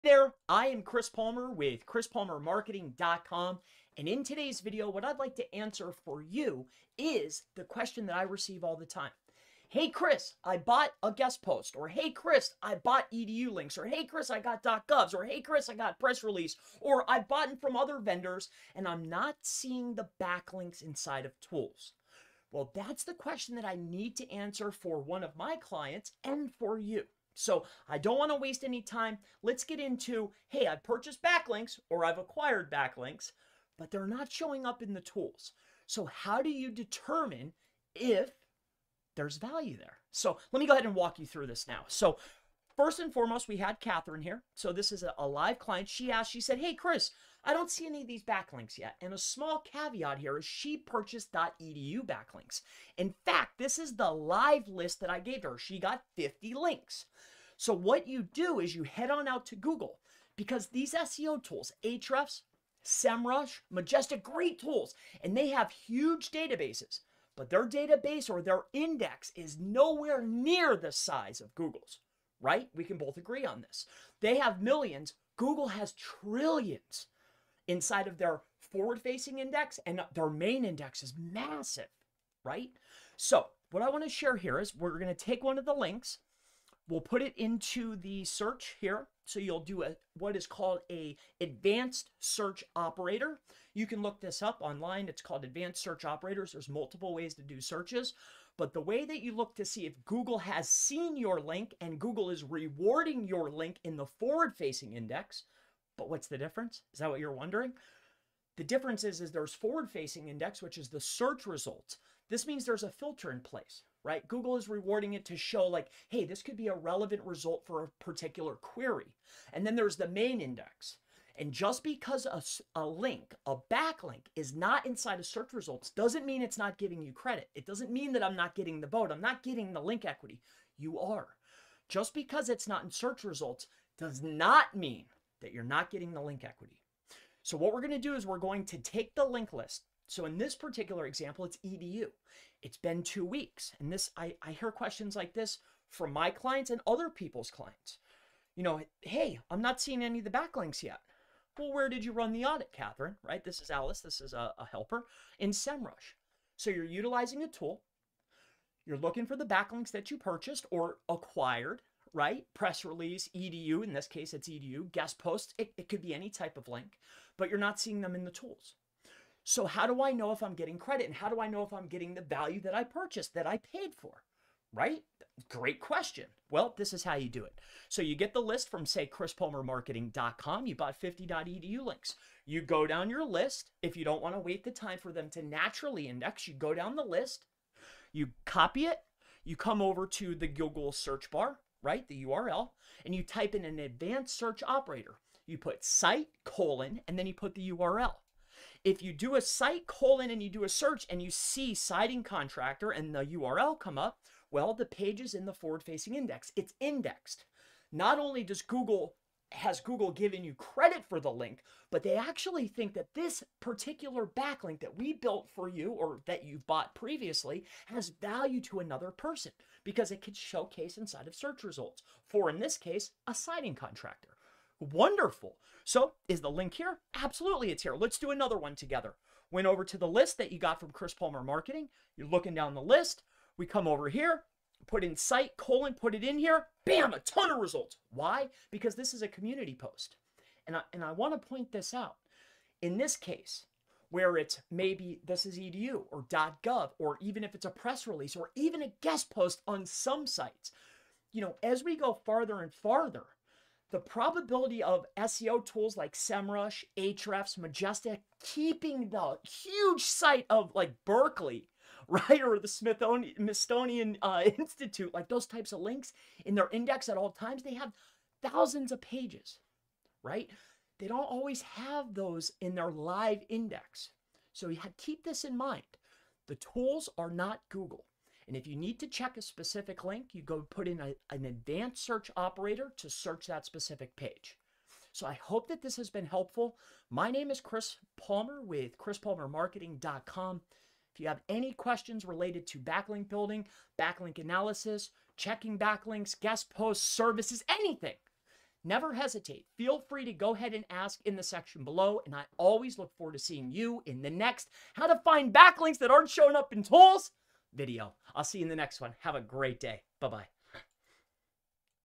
How to Check Backlinks Not Indexing in SEO Tools
In today's video what I will walk you through is exactly how you can check backlinks that aren't showing up inside of your SEO tools.

Just because a SEO tool like Ahrefs, SEMrush or Majestic SEO tools is not showcasing your recently acquired backlink from your recent link building. Will not indicate that the backlink is not live or valuable to you or your business.

We have to remember that SEO tools are not Google and the search results are not the index this video will help you decipher the differences between search results and ranking along with that how you can find backlinks that are actually indexed and helping your business.

If you have any questions related to how to check backlinks that aren't indexing in SEO tools please feel free to go ahead and ask below.

Chris Palmer Marketing SEO
 PA 18252 USA